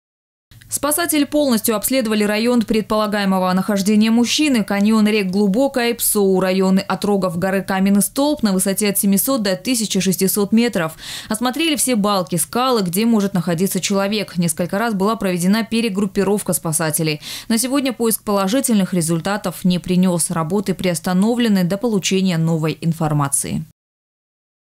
Работы по поиску сноубордиста в Сочи временно приостановлены
На протяжении трёх недель спасатели ЮРПСО МЧС России вели работы на южном склоне хребтаАибга по поиску пропавшего сноубордиста из Москвы.

Спасатели полностью обследовали район предполагаемого нахождения мужчины — каньоны рек Глубокая и Псоу, районы отрогов г.Каменный Столб на высоте от 700 до 1600 метров, осмотрели все балки, скалы, где может находиться человек. Несколько раз была проведена перегруппировка спасателей. На сегодня поиск результатов не принес. Работы приостановлены до получения новой информации, сообщает пресс-служба ЮРПСО МЧС России.

Сочи. Новости.
Телеканал Эфкате.